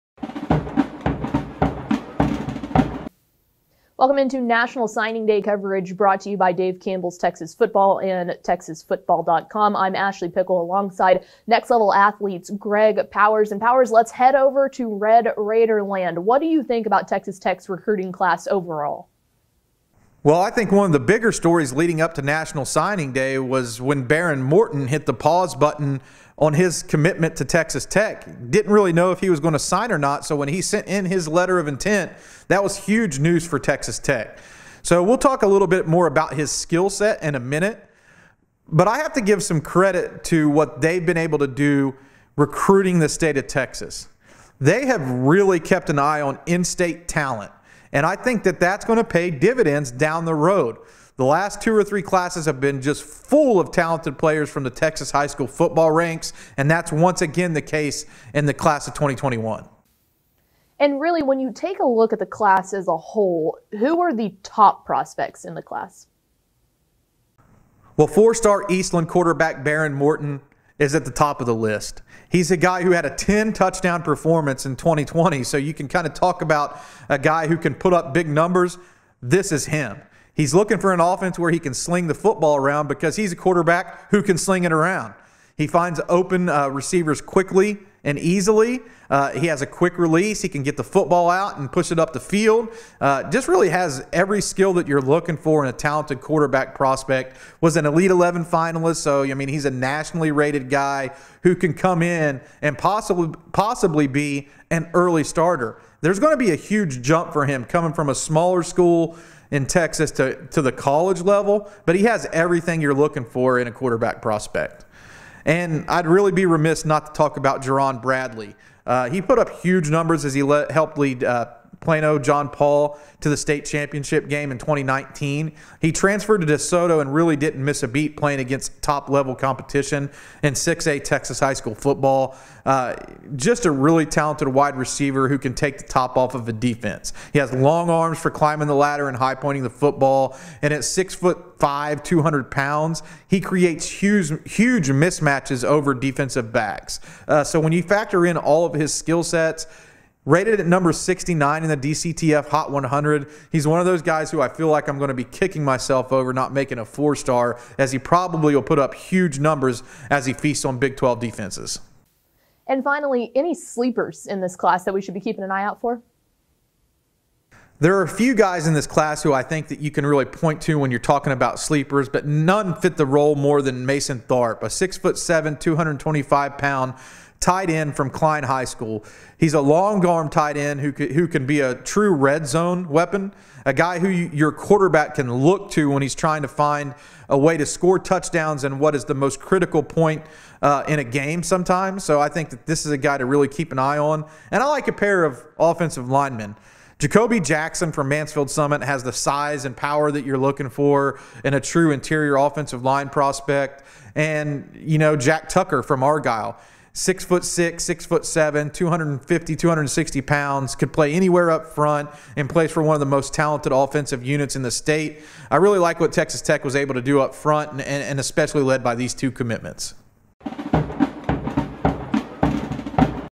Early Signing Period Central: Texas Tech Red Raiders
TexasFootball.com takes a closer look at Texas Tech's 2021 recruiting class with video analysis and a breakdown of the top prospect names to know.